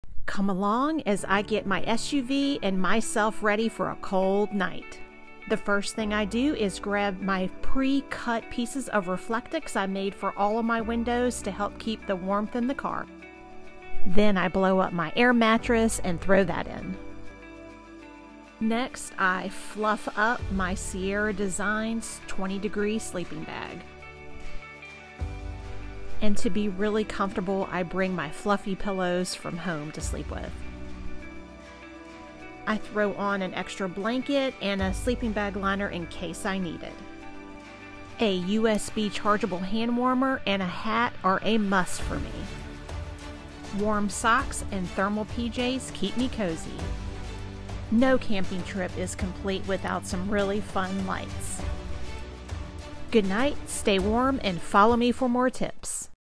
🛏Sleeping in my Ford Escape on a 30 Degree Night
Come with me as I set up my car for sleeping on a cold winter night.

🌟Other ways to show your SUPPORT:

Hit the THANKS button under the video to leave a SUPER THANKS DONATION.